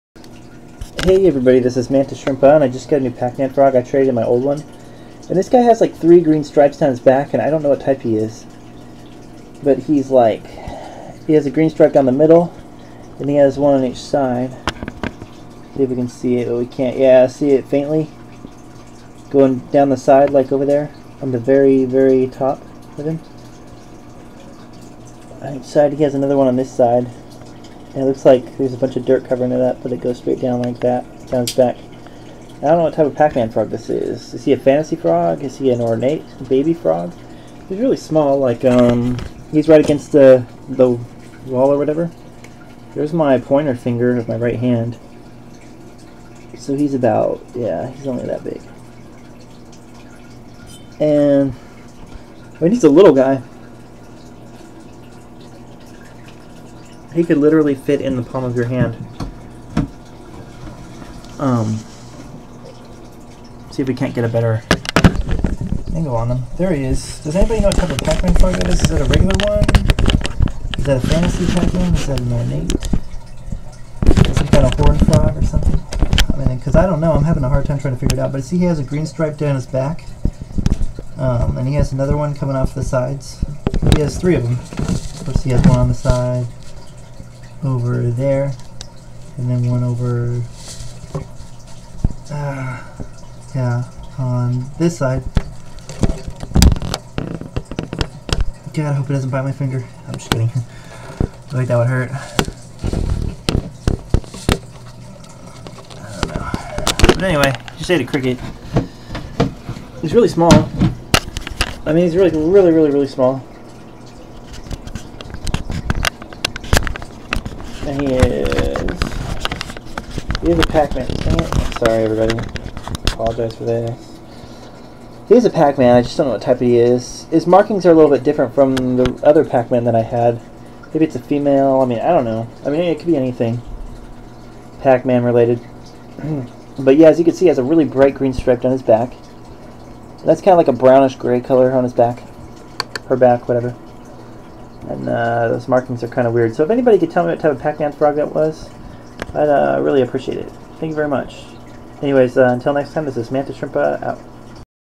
pacman frog
I dont know what type of pacman this is exactly it might be a fantasy pacman or an ornate or regular